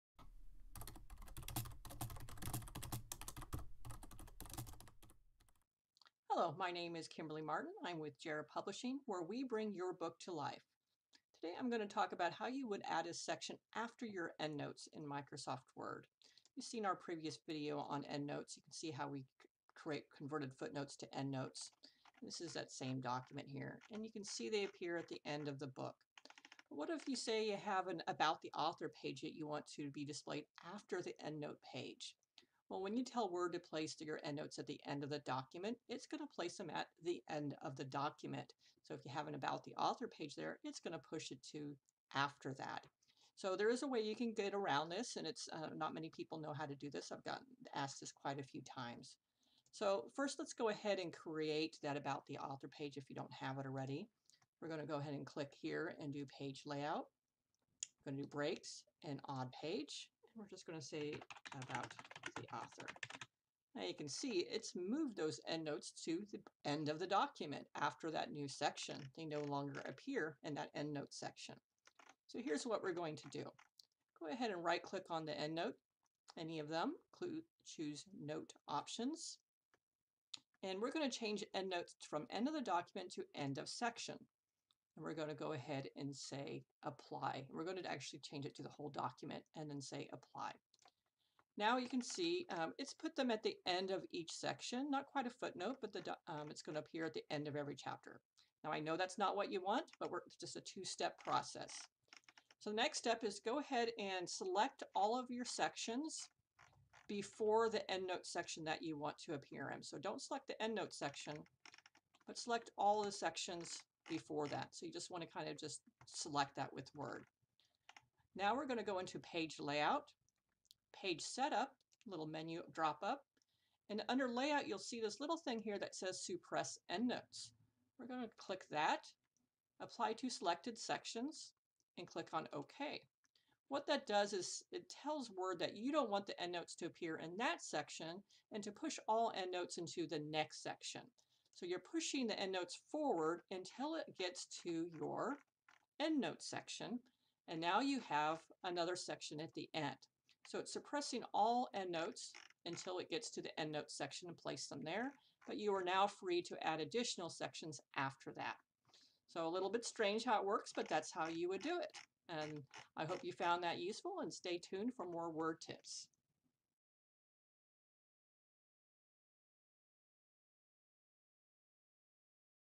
Word Tips for Writers: Add a Section After Your Endnotes in Microsoft Word
How to add a section following your endnotes in the back of your book's manuscript in Microsoft Word. By Kimberly Martin of Jera Publishing.